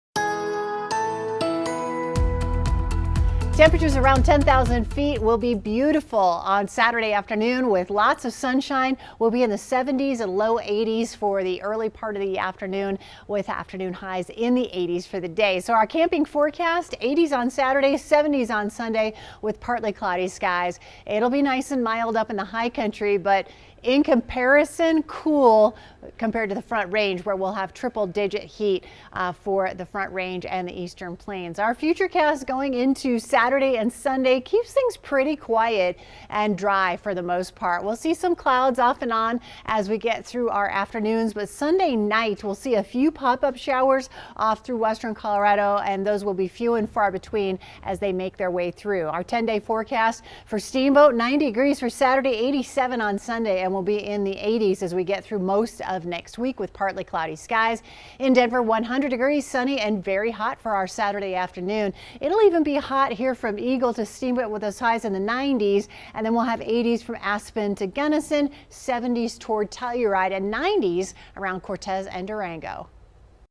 As triple-digits loom in Denver this weekend, it stays cool in the high country
Escape the heat this weekend in the mountains, where pleasant and partly cloudy conditions are expected. Stacey Donaldson has the forecast (July 8, 2022)